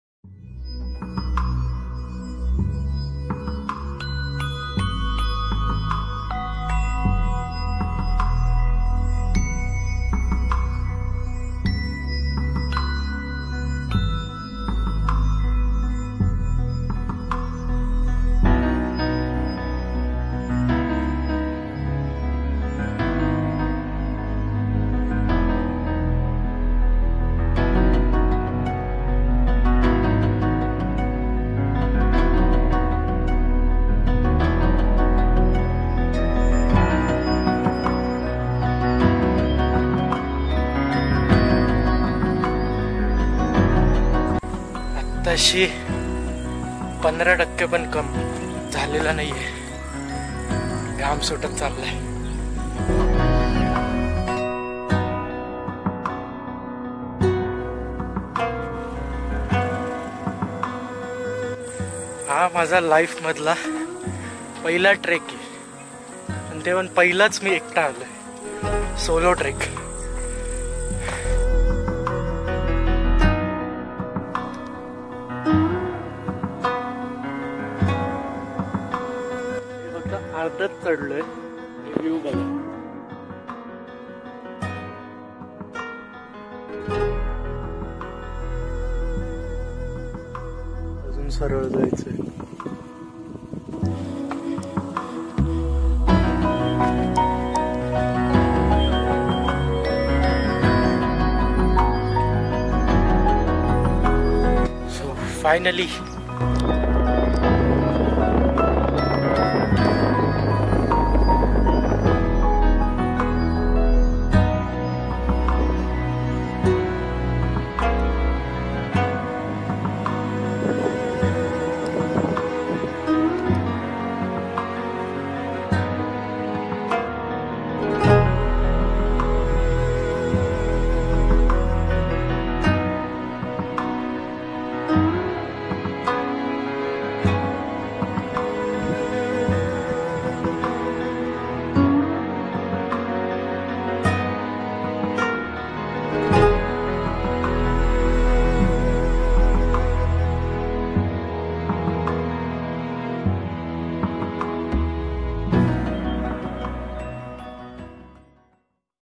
KalsuBai Peak | Sahyadri Mountain Range | Cinematic | Danny-l Films
कळसुबाई!

Kalsubai Peak.

Description

Kalsubai is a mountain in the Western Ghats, located in the Indian state of Maharashtra. Its summit, situated at an elevation of 1,646 metres, is the highest point in Maharashtra. The mountain range lies within the Kalsubai-Harishchandragad Wildlife Sanctuary.